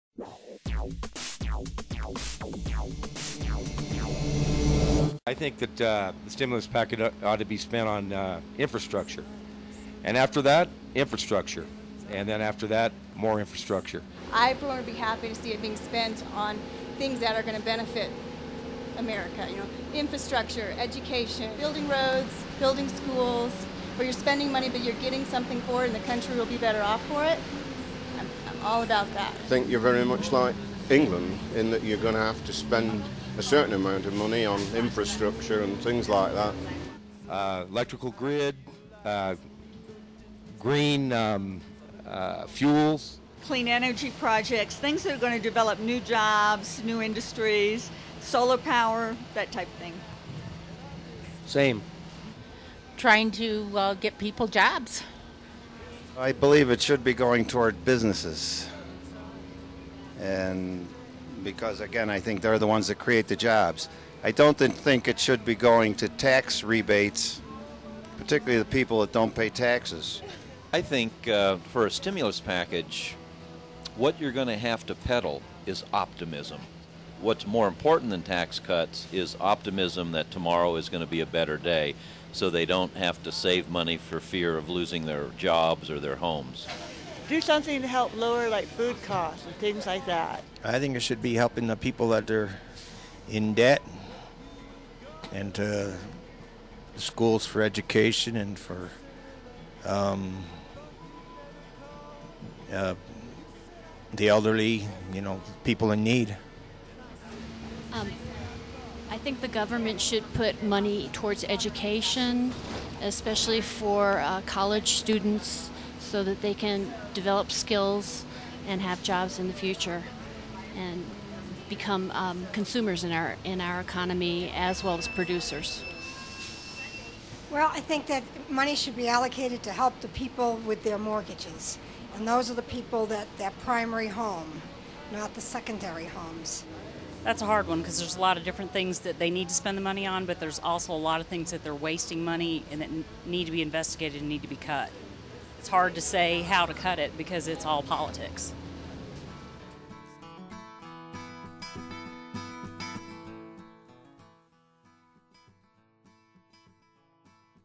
Voices in the Street_Stimulus package
"Where do you believe government stimulus money should be allocated?"